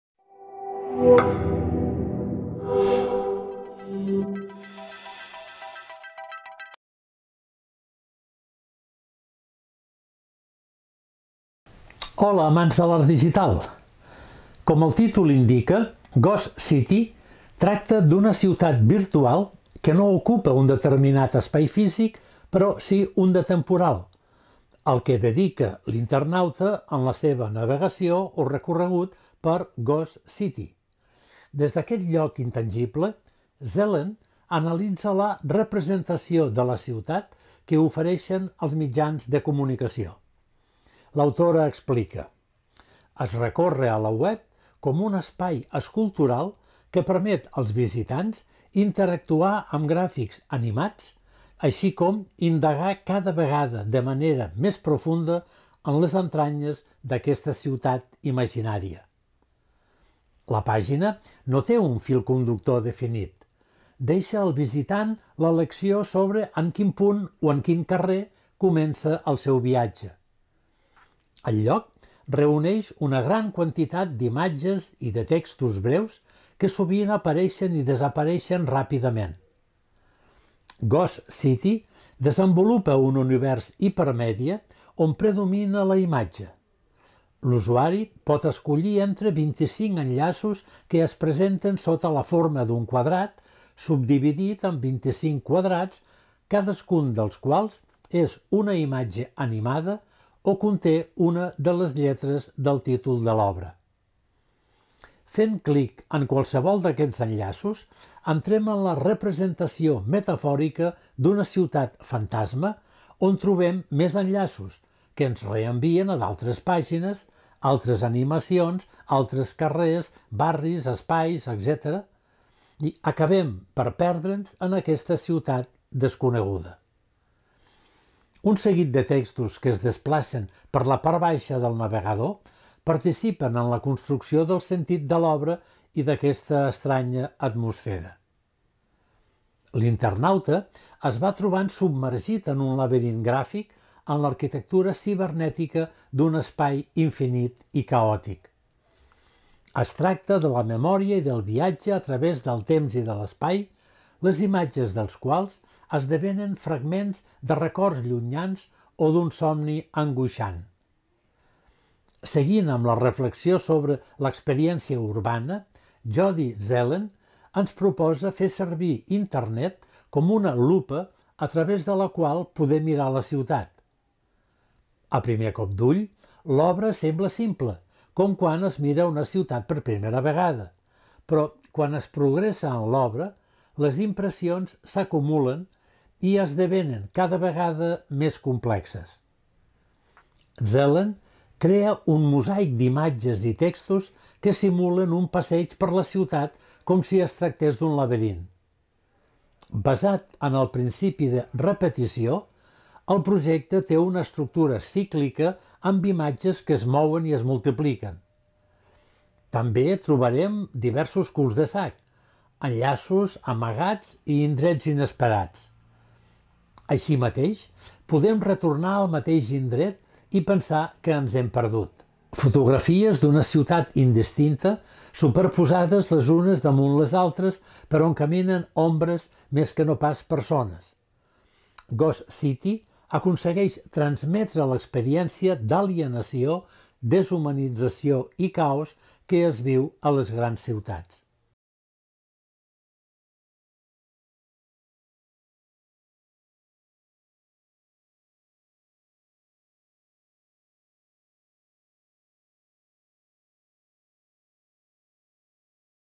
Curs: Art digital i Net.art. Jody Zellen: Ghost city
Ghost City és un lloc web centrat en la representació de la ciutat per part dels mitjans de comunicació de masses.

Va crear la pàgina web Ghost City durant la seva estada com a artista resident al Banff Centre for the Arts d'Alberta (Canadà)

Es tracta d'un entorn laberíntic pel qual l'espectador pot navegar, ja sigui seguint la narració lineal que es desenvolupa passant d'una pàgina a una altra, o bé endinsant-se en el caos no lineal d'enllaços aleatoris.

Cada espai està format per imatges i textos apropiats. Les imatges procedeixen de diversos mitjans impresos. Els textos són passatges de teoria urbana o reflexions poètiques escrites específicament sobre la ciutat.

En lloc de presentar imatges estàtiques, Ghost City és un collage de peces en moviment. És una retícula d'imatges parpellejants que es repeteixen indefinidament.

L'espectador és un rodamon urbà que es mou pel lloc, pas a pas, pàgina a pàgina. Un avança i recula sobre els seus propis passos dins la quadrícula urbana, descobrint nous espais i nous significats.

Es fa clic en una paraula: «passeig», «temps», «espai» o a la imatge d'una silueta, una ombra o una façana. El moviment es congela, un color parpelleja i un es troba en un altre lloc.

Una a una apareix una graella d'imatges nova. Algunes em resulten familiars. He estat aquí abans?

Ghost City tracta sobre la memòria i sobre viatjar a través del temps i l’espai. El temps és infinit. L’espai és finit. És el ciberespai. Tot i això, el temps i l'espai de Ghost City es relacionen metafòricament amb l'experiència de la ciutat, on la gent camina, parla i interactua.

Dins dels confins de Ghost City, els visitants poden aturar-se a pensar, avançar i retrocedir. Ghost City és una ciutat de fragments. Un record. Un fantasma de la realitat. Una ciutat fantasma.